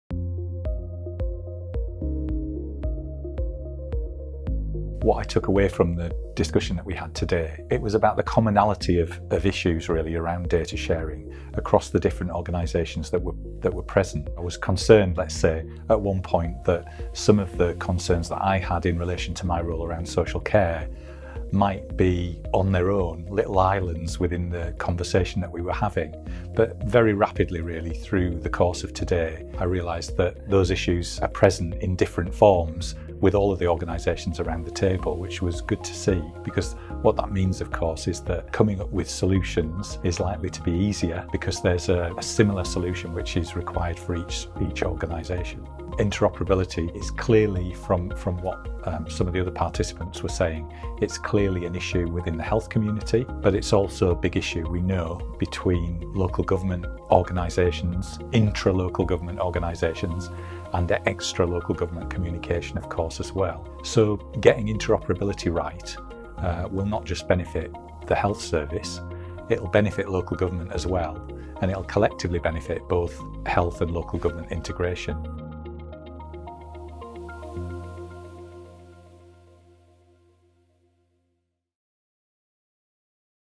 HSJ/LGC roundtable on data sharing – part two
NHS Digital's Mark Nicholas discusses 'the commonality of issues' among the health community regarding data sharing, and how different sectors have similar goals and need not fear they are working at cross purposes